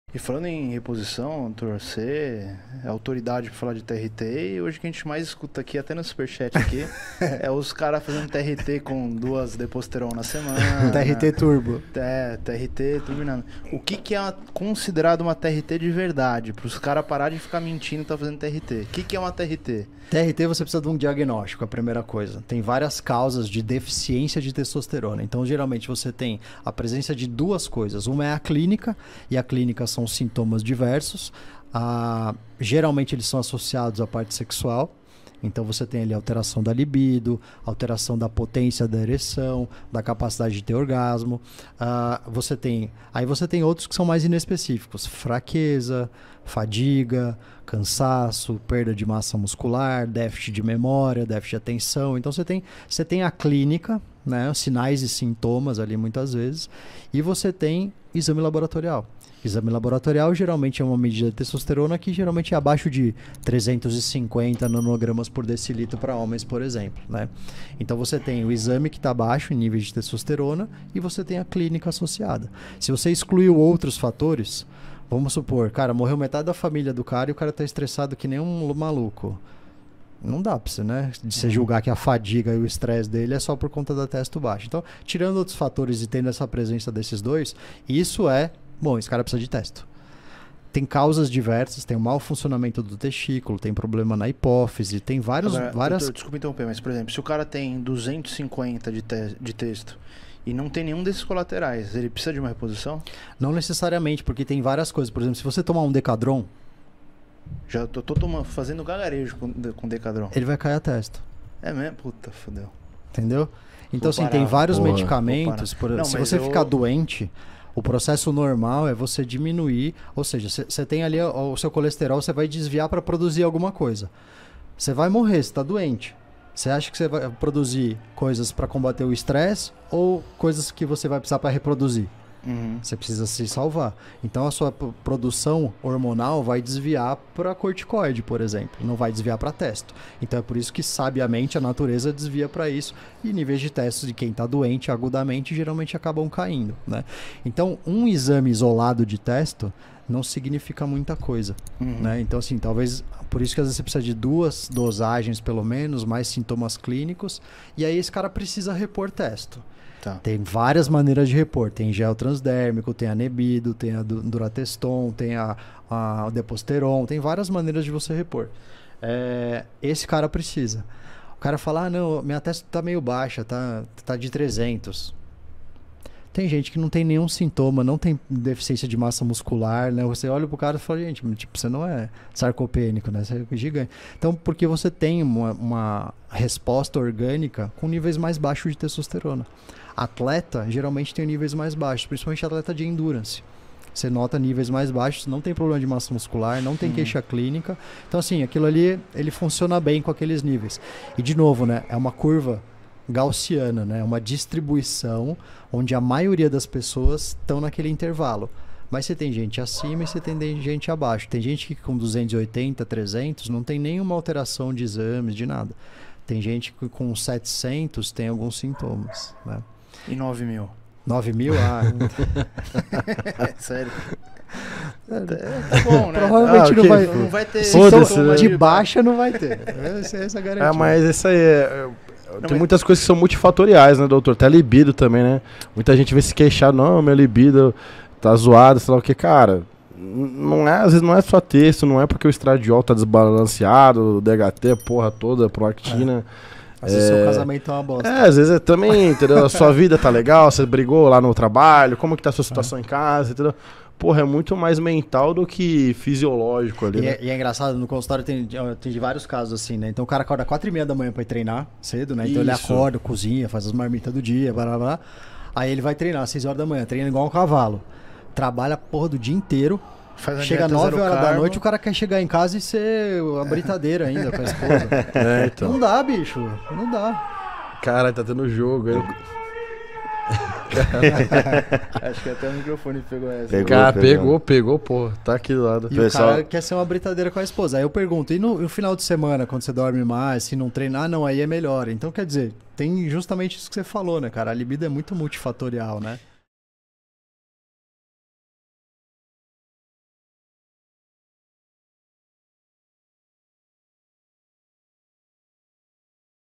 COMO SABER SE VOCÊ PRECISA FAZER REPOSIÇÃO DE TESTOSTERONA? TRT PARA NATURAL COM BAIXA TESTO
DR. LUCAS CASERI - REVOLUÇÃO ANABÓLICA!